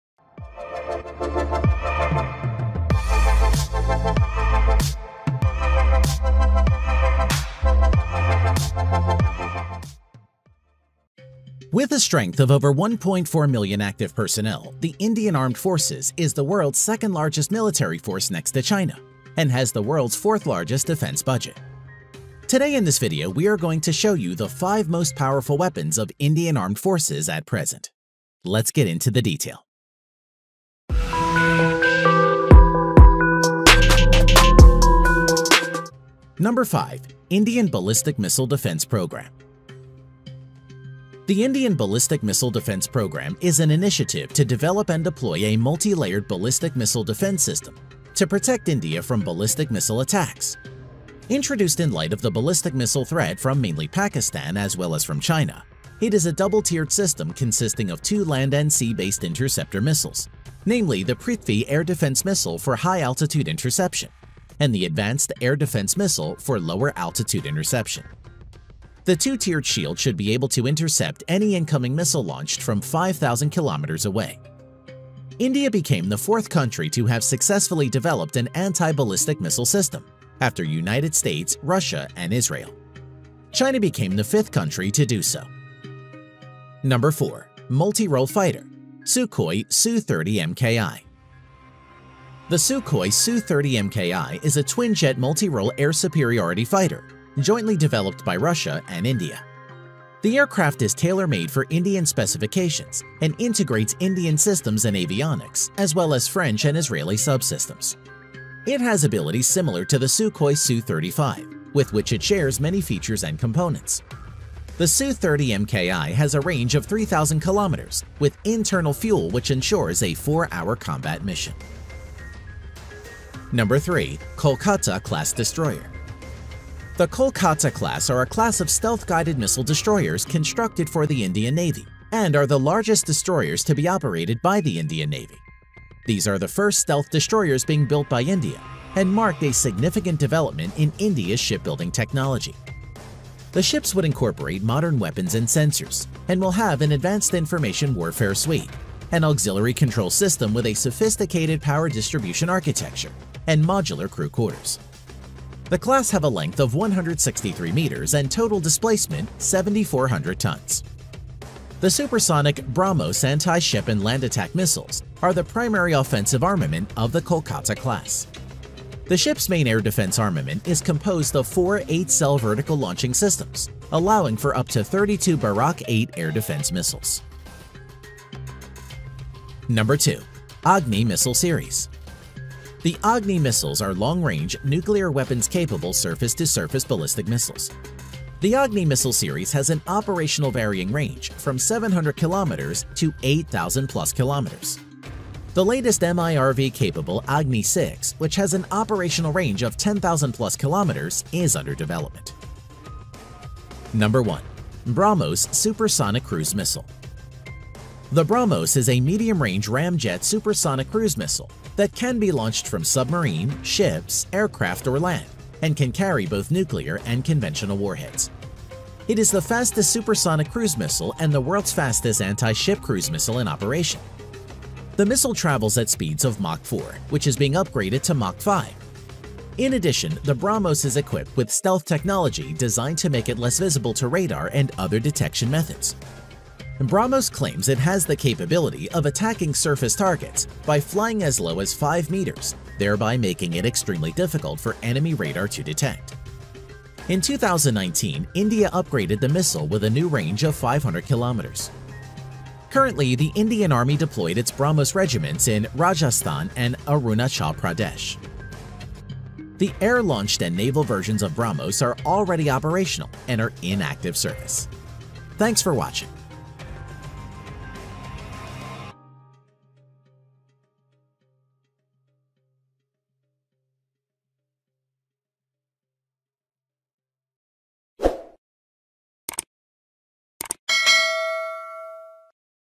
Top 5 most powerful weapons of India - Weapons used In 2019 - Must see
5 Most Powerful Weapons Of India At Present.
With the strength of over 1.4 million active personnel,the Indian Armed Forces is the world's second-largest military force next to China,and has the world's fourth-largest defence budget.
Today in this video we are going to show you the 5 most powerful weapons of Indian Armed forces at present.

*NUMBER 5..Indian Ballistic Missile Defence Programme

The Indian Ballistic Missile Defense Programme is an initiative to develop and deploy a multi-layered ballistic missile defence system to protect India from ballistic missile attacks.Introduced in light of the ballistic missile threat from mainly Pakistan, as well as from China, it is a double-tiered system consisting of two land and sea-based interceptor missiles, namely the Prithvi Air Defese  missile for high altitude interception, and the Advanced Air Defence Missile for lower altitude interception.

The two-tiered shield should be able to intercept any incoming missile launched from 5,000 kilometres away.

India became the fourth country to have successfully developed an anti-ballistic missile system, after United States, Russia, and Israel. China became the 5th country to do so.

*NUMBER 4...MULTIROLE FIGHTER Sukhoi Su-30MKI

The Sukhoi Su-30MKI is a twinjet multirole air superiority fighter jointly developed by Russia and India.The aircraft is tailor-made for Indian specifications and integrates Indian systems and avionics as well as French and Israeli sub-systems.It has abilities similar to the Sukhoi Su-35 with which it shares many features and components.

The Su-30MKI has a range of 3,000 km with internal fuel which ensures a 4 hour combat mission.

*NUMBER 3... KOLKATA CLASS DESTROYER

The Kolkata class  are a class of stealth guided missile destroyers constructed for the Indian Navy  and are the largest destroyers to be operated by the Indian Navy.These are the first stealth destroyers being built by India and marked a significant development in India's shipbuilding technology.

The ships would incorporate modern weapons and sensors, and will have an advanced information warfare suite, an auxiliary control system with a sophisticated power distribution architecture, and modular crew quarters.

The class have a length of 163 m and total displacement 7400 tonnes.

The supersonic BrahMos anti-ship and land-attack missiles are the primary offensive armament of the Kolkata class.

The ship's main air-defence armament is composed of four eight-cell vertical launching systems allowing for up to thirty-two Barak 8 air defence missiles.

*NUMBER 2...AGNI MISSILE SERIES

The Agni missiles are long range, nuclear weapons capable surface to surface ballistic missile. The Agni missile series has a  operational range varying from 700km to 8000+ km.The latest MIRV Capaple Agni 6 which has a operational range of 10,000+ km is under development.

*NUMBER 1..BRAHMOS SUPERSONIC CRUISE MISSILE

The BrahMos is a medium-range ramjet supersonic cruise missile that can be launched from submarine, ships, aircraft, or land and can carry both nuclear and conventional warhead.

 It is the fastest supersonic cruise missile and the world's fastest anti-ship cruise missile in operation.

The missile travels at speeds of Mach 4,which is being upgraded to Mach 5.In addition, the BrahMos is equipped with stealth technology designed to make it less visible to radar and other detection methods.BrahMos claims it has the capability of attacking surface targets by flying as low as five meters thereby making it extremely difficult for enemy radar to detect.

In 2019, India upgraded the missile with a new range of 500 km.

Currently the indian army deployed its brahmos regiments in

Rajasthan and Arunachal pradesh.

The air-launched and naval versions of brahmos are already operational and are in active service.